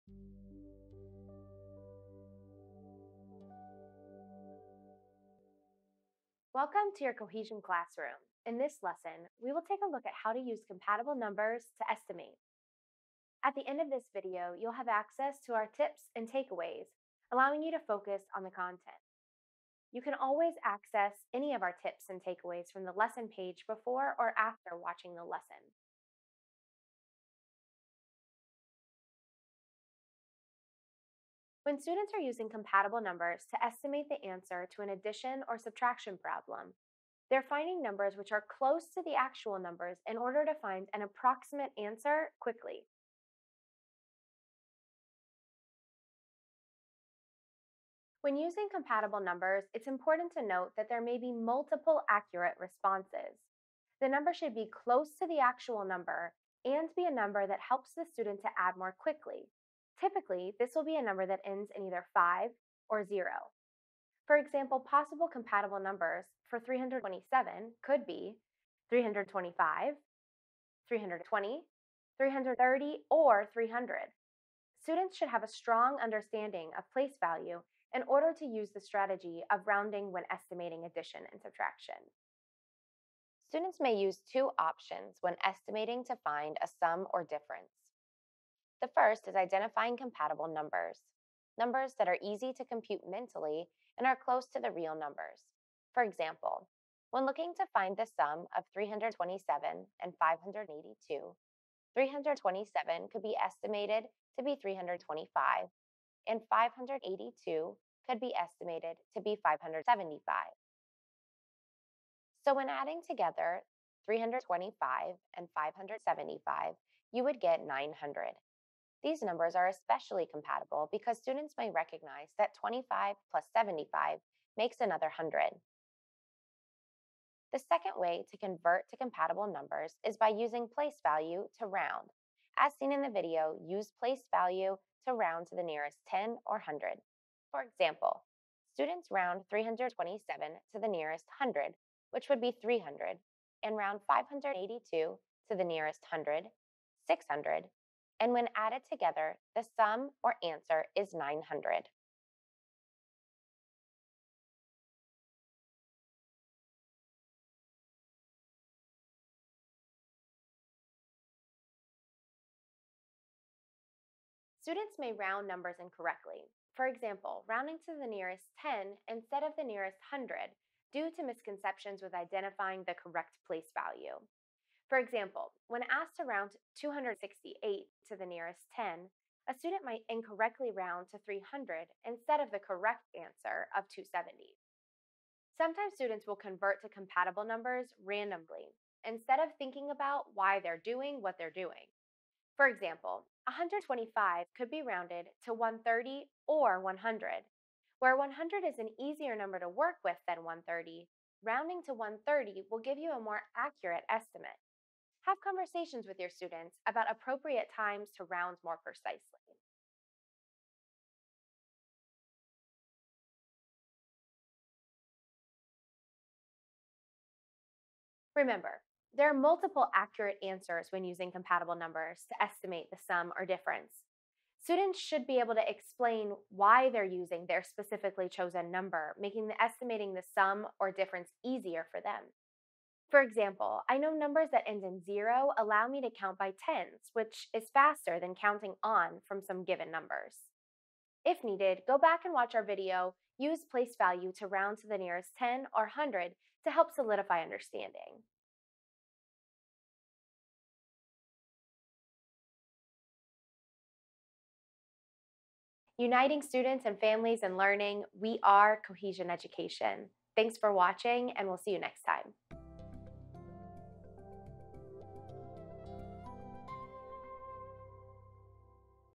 Use Compatible Numbers to Estimate
When estimating to find the sum or difference, students have 2 options; 1) Identify Compatible Numbers, 2) Use Place Value to Round. Compatible numbers are close to the actual numbers, but are easy or friendlier to work with. Students need practice in making appropriate and strategic choices in rounding or selecting compatible numbers.